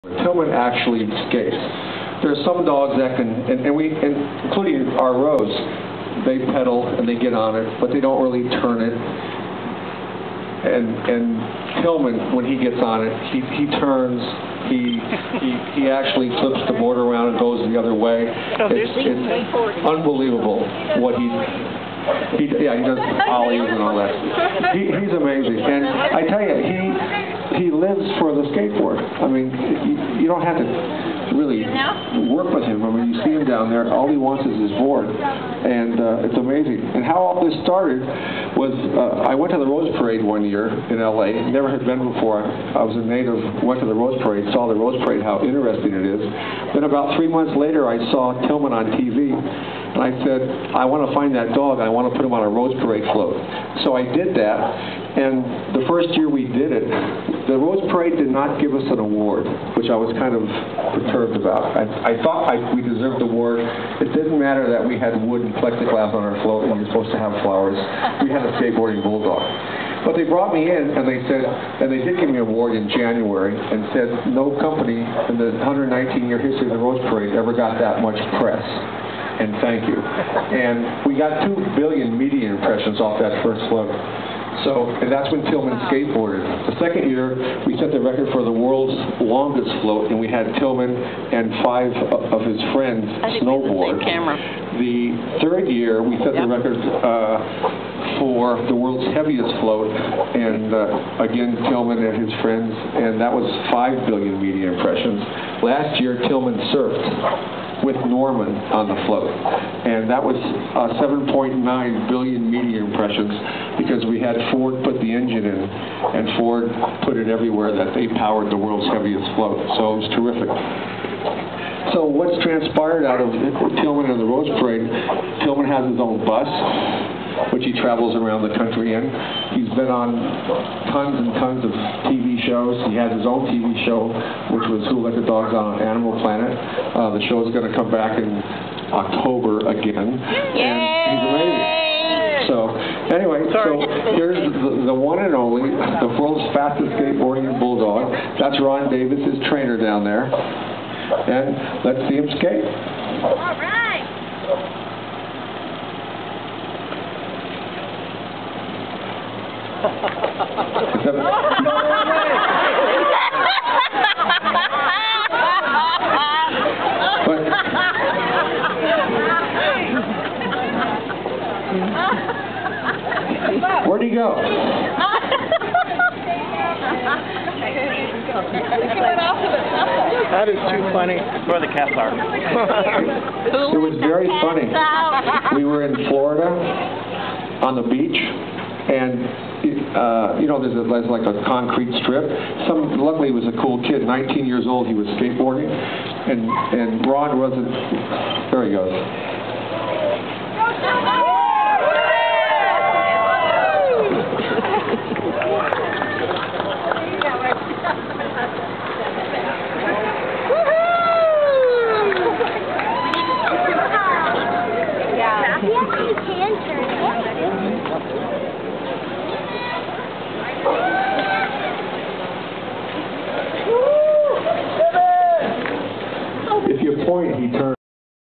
Tillman on his skateboard
Tillman rides his skateboard at BlogPaws 2012